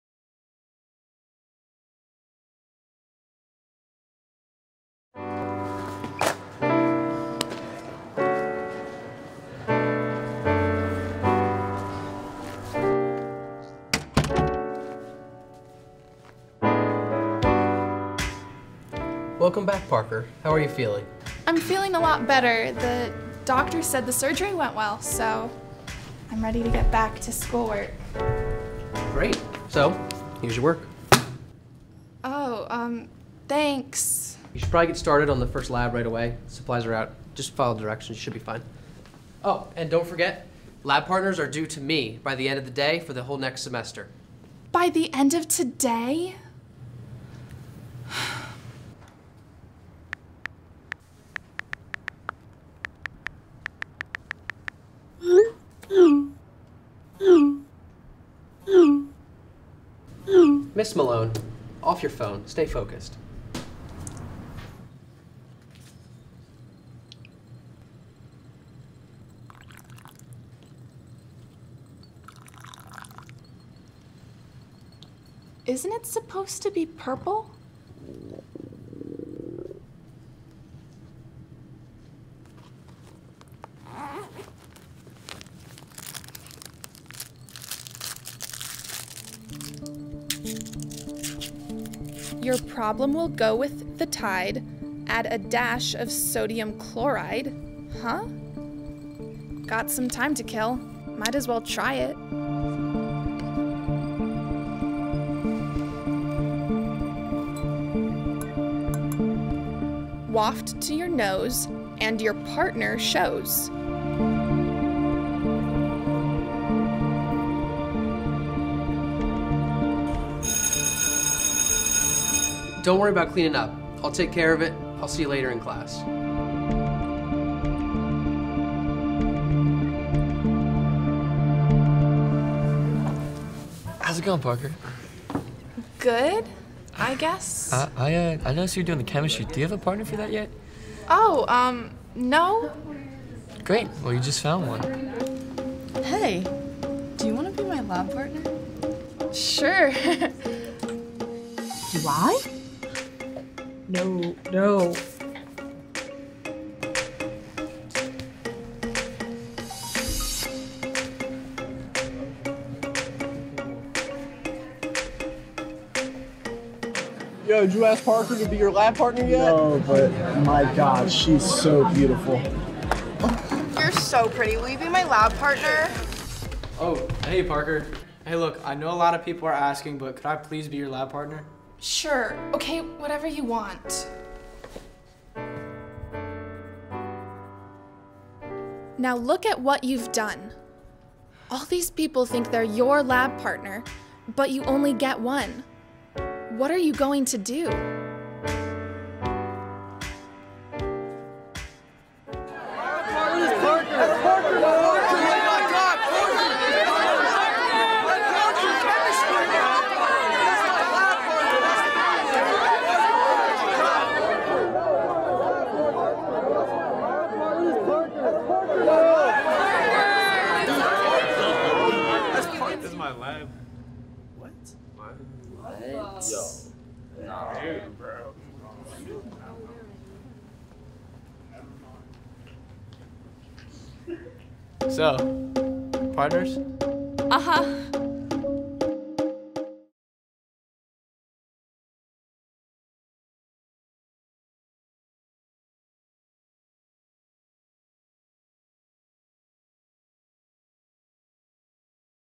Spring-Ford 10-Day Film Challenge | Pure Chemistry
Nominated For: Best Use of Prop, Best Editing, Best Screenplay and Best Use of Character

Won: Best Use of Prop, Best Screenplay and #1 Overall in THE WORLD.

Congratulations McKinley Linn, Sarah Camilleri, Nick Elsner, Dana Reed, Emma Grohe and Emma Begg